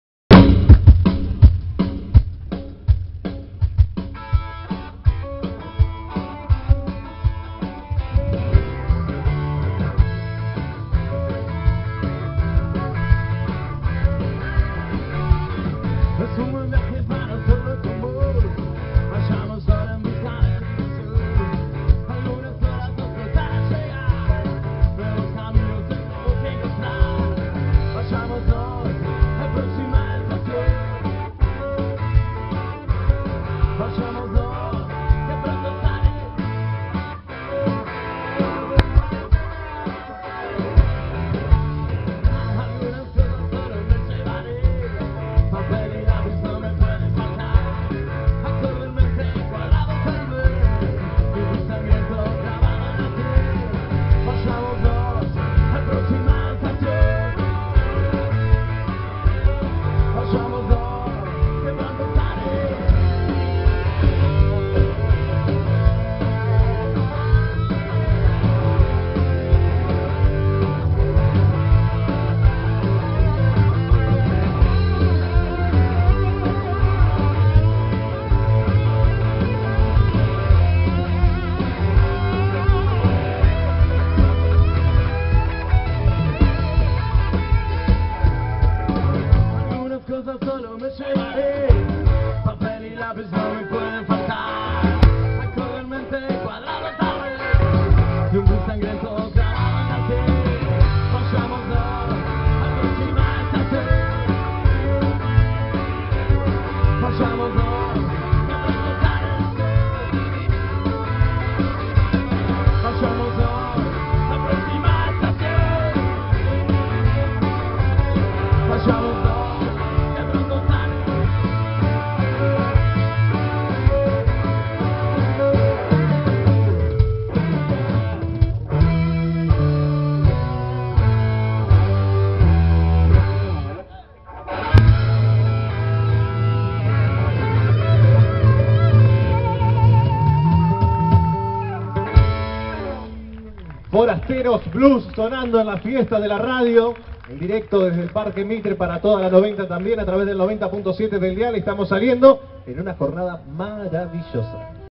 Forasteros Blues - Parque Mitre (Video 8) - Olavarría - 21/Septiembre/2016
Show de Dia de la Primavera con la actuación de Forasteros Blues en el Parque Mitre de Olavarría el 21 de Septiembre de 2016.

Seguinos en :
Rock Nacional en tu Idioma